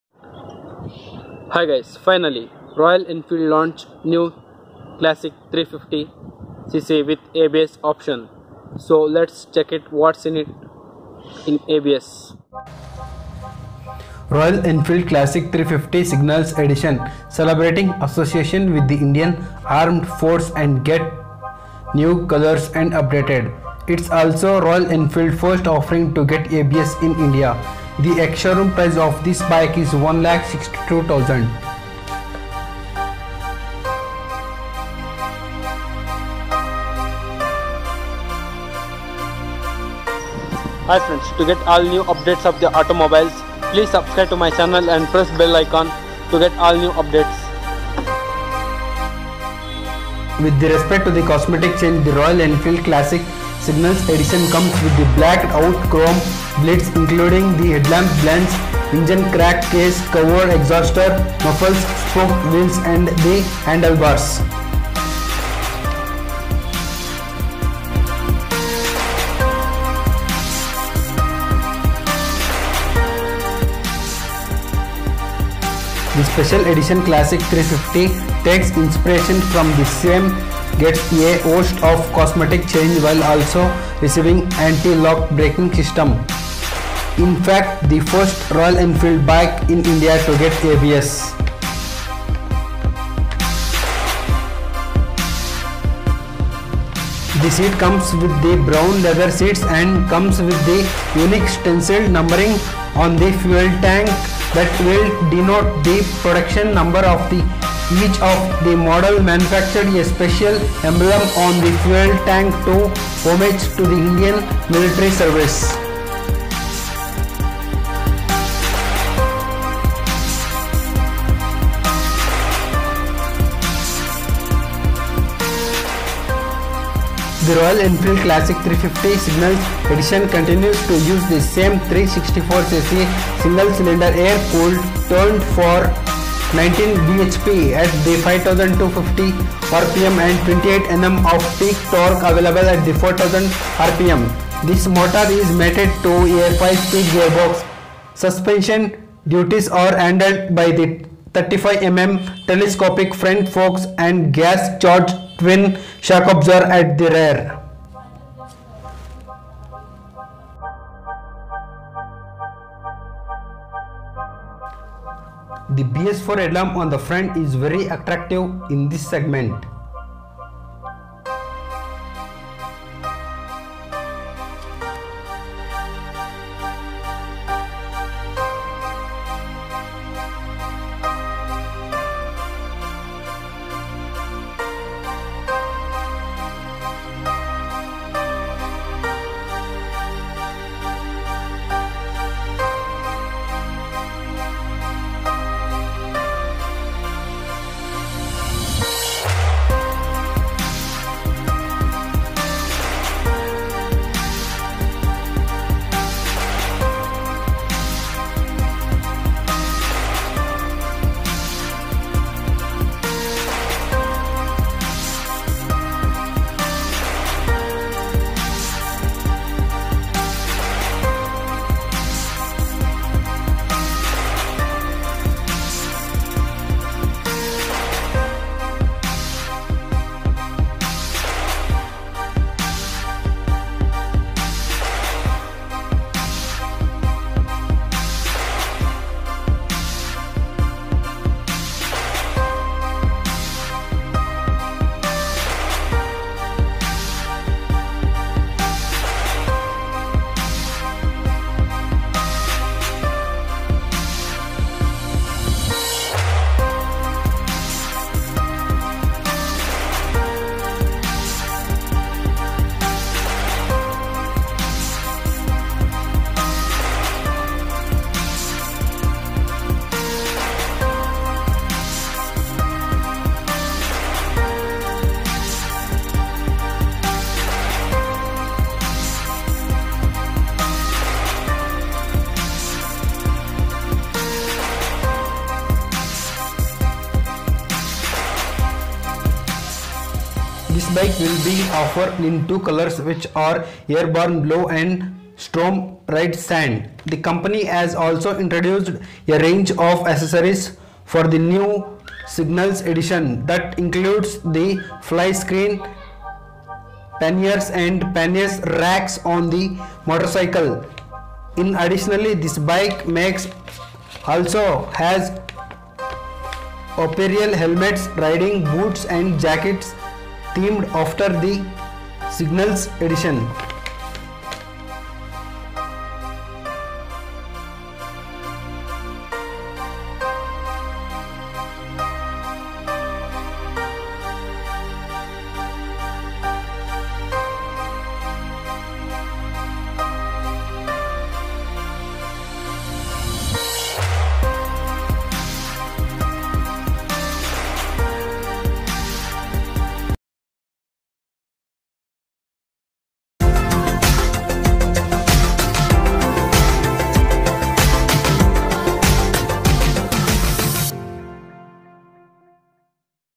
Royal Enfield Classic 350 ABS signals New 2018 walk around
Introducing a new special edition version in the Classic series, Royal Enfield has launched the Classic 350 Signals Edition in India priced at Rs. 1.62 lakh (ex-showroom, Pune). The new Royal Enfield Classic 350 Signals Edition celebrates the motorcycle maker's 65-year-old association with the Indian Armed Forces, specifically the Indian Army Corps of Signals. The special edition Classic 350 takes inspiration from the same gets a host of cosmetic changes, while also receiving Anti-Lock Brakes (ABS) as standard. It is in fact, the first Royal Enfield bike in India to get ABS. Bookings for the Classic 350 Signals Edition have begun across Royal Enfield dealerships, while deliveries will commence sometime next month. The Signals Edition is priced at a premium of approximately Rs. 15,000 over the standard model.

Also Read: Royal Enfield To Launch Limited Edition Classic 350 In India

Royal Enfield has tried to convey another story from its long legacy in India, having originally been a supplier to the army for service motorcycles since 1952. The Indian Army Corps of Signals is the division that overseas military communications for the armed forces. The company says that the Signals Edition pays tribute to not only the armed forces but also the men and women in the sky and at the borders that ride these motorcycles.With respect to cosmetic changes, the Royal Enfield Classic Signals Edition comes with blacked out chrome bits including the headlamp bezel, engine, crankcase cover, exhaust muffler, spoked wheels, and the handlebar. The bike is available in brown leather seats for the rider and pillion, which complements both colour options. The bike also comes with the unique stencilled numbering on the fuel tank that will denote the production number for each of the models manufactured. The bike maker has also incorporated a special emblem on the fuel tank to homage to the Indian military's services.Mechanically, the Royal Enfield Classic 350 Signals Edition continues to use the same 346 cc single-cylinder, air-cooled tuned for 19 bhp at 5250 rpm and 28 Nm of peak torque available at 4000 rpm. The motor is mated to a 5-speed gearbox. Suspension duties are handled by 35 mm telescopic front forks and gas-charged twin shock absorbers at the rear, while braking performance comes from a single disc at the front and at the rear as well.
The good part is that the motorcycle also gets ABS as standard and is a first for any bike in the Royal Enfield portfolio. The bike will be offered in two colours which are Airborne Blue and Stormrider Sand. The company has also introduced a range of accessories for the new Signals Edition that includes the flyscreen, panniers and pannier racks on the motorcycle (pictured above). In addition, the bike maker also has apparel, helmets, riding boots and jackets themed after the Signals edition.